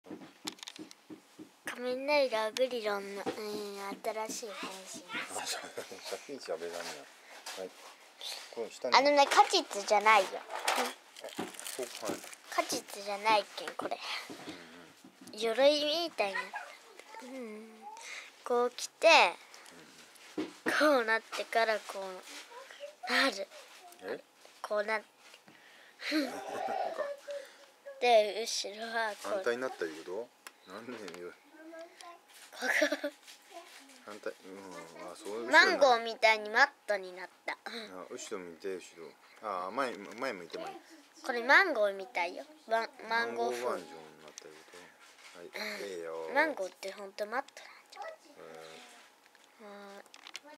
仮面ライダーグリドン新アームズ作成８才コウキ
２０１４年１月３１日（金）撮影
これの方がカッコ良くて変身させ易いらしい(*^_^*)
マンゴー風らしい(~o~)

【新発売】仮面ﾗｲﾀﾞｰ『ｸﾞﾘﾄﾞﾝ&ﾀﾞﾝﾃﾞﾗｲﾅｰｾｯﾄ』ｱｰﾑｽﾞﾁｪﾝｼﾞｼﾘｰｽﾞ07⑦
↑現在約４００回再生。。。まあ、こんなもんでしょ～う(^_^;)